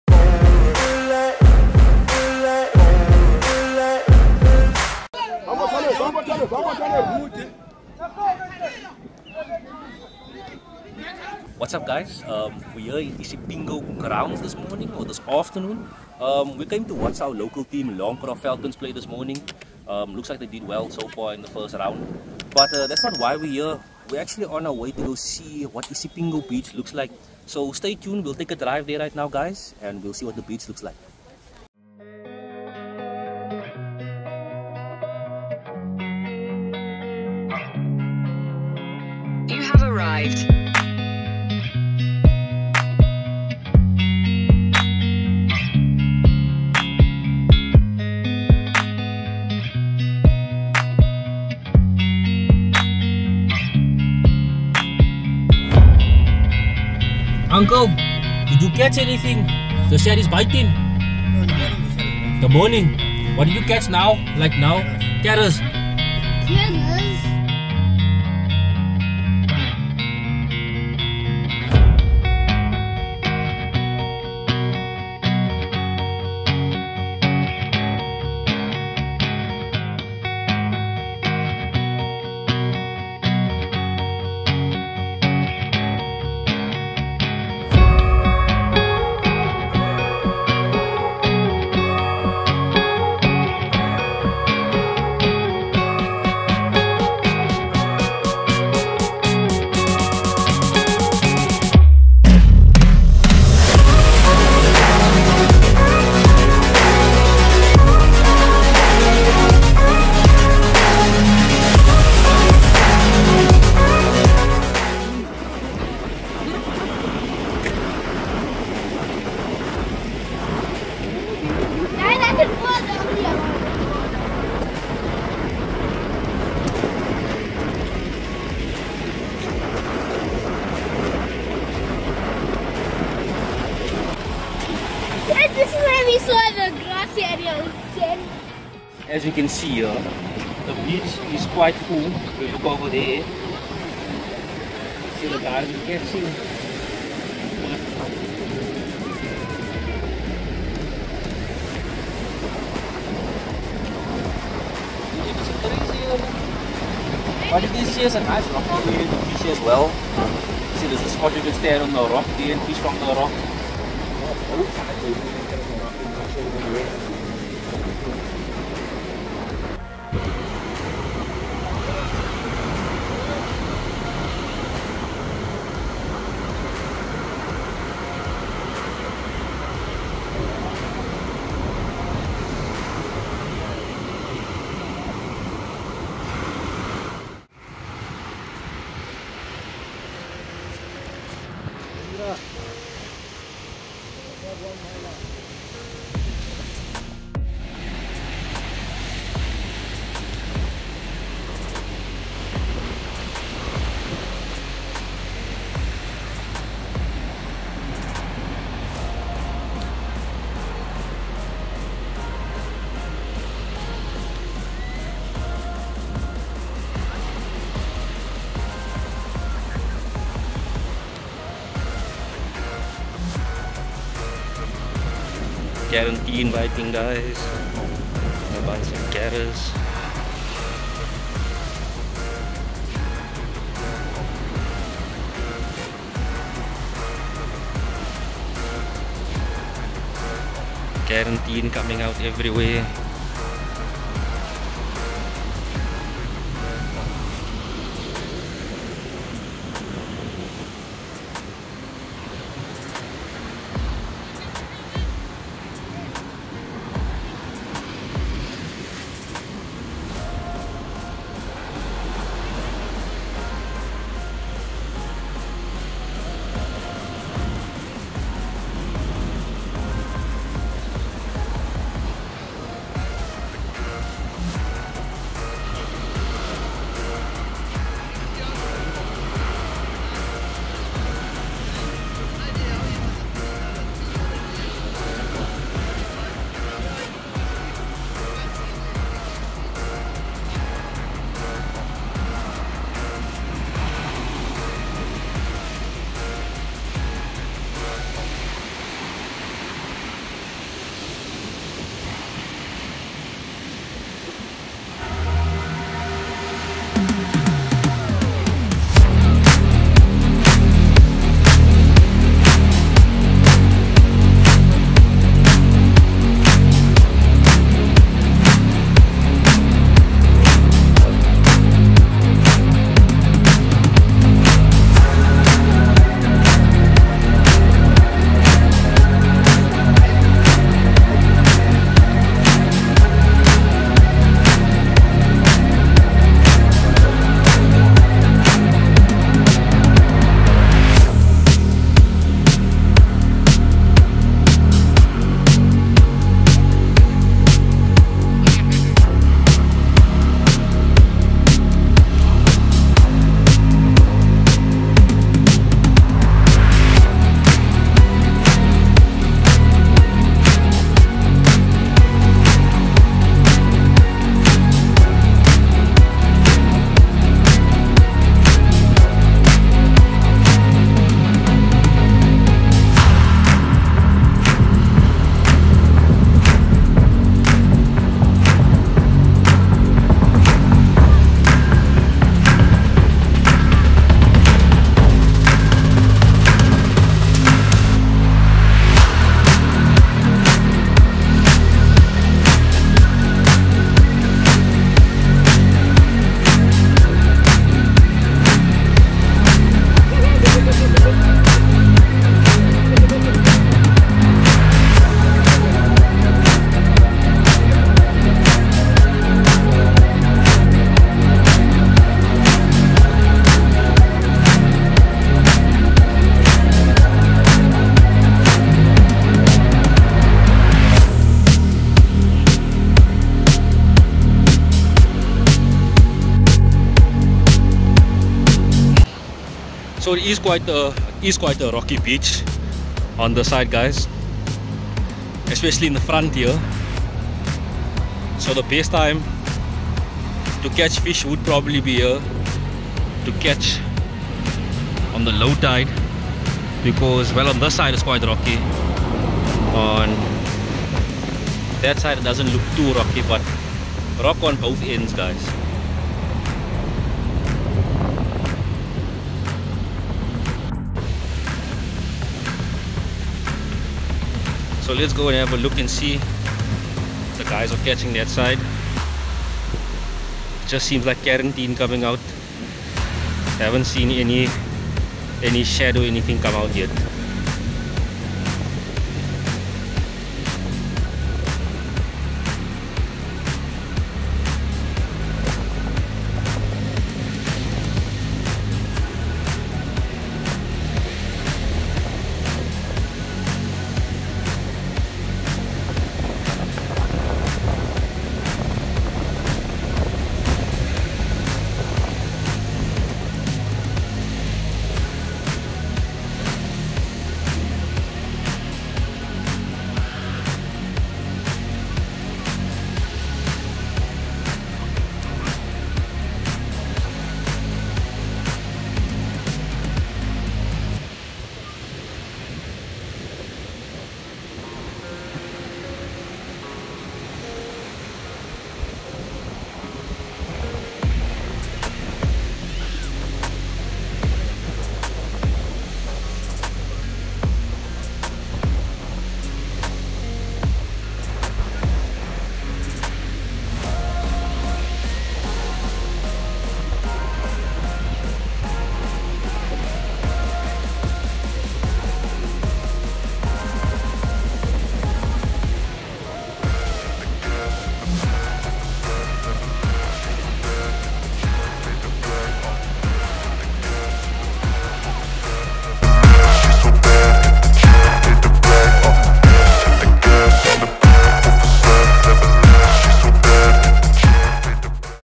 Checking out fishing at isipingo beach after
After watching some local soccer matches in the Nedbank cup we just poped over the hill to see how the fishing was yesterday in isipingo.